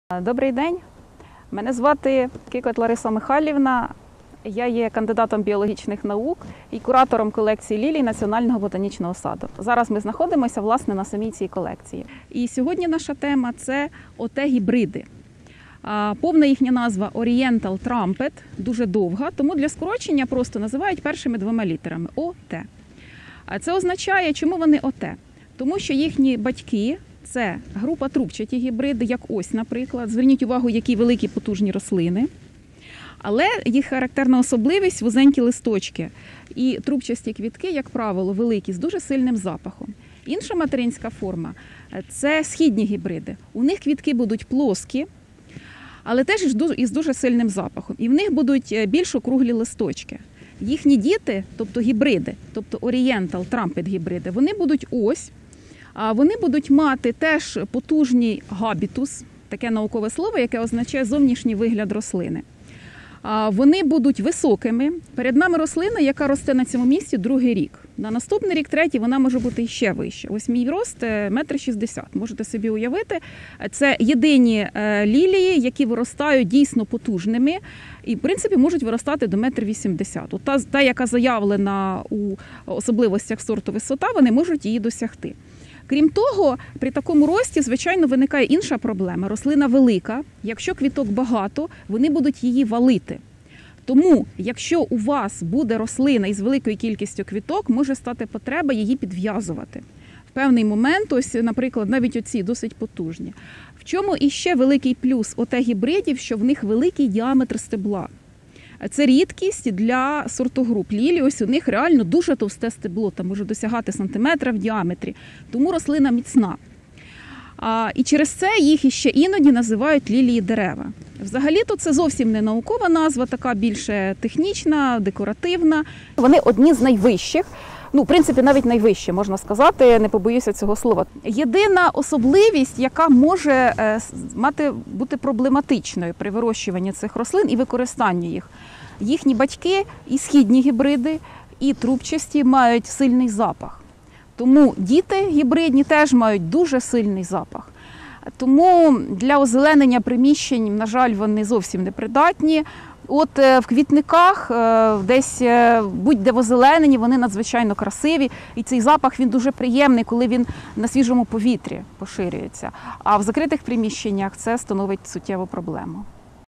Як виростити лілійне дерево? 1️⃣ ОТ-гібриди: розміри, аромат, краса! 🌺🌿
Лілійні дерева (ОТ-гібриди) 🌺🌸🌼 мають великі розміри, неперевершену красу, виражений аромат! З року в рік виростають все вищими! Це єдині лілії, які виростають настільки високими!

🔴 Детальніше розповідає Лариса Кикоть, кандидат біологічних наук, куратор колекції лілій Національного ботанічного саду ім.М.М.Гришка НАН України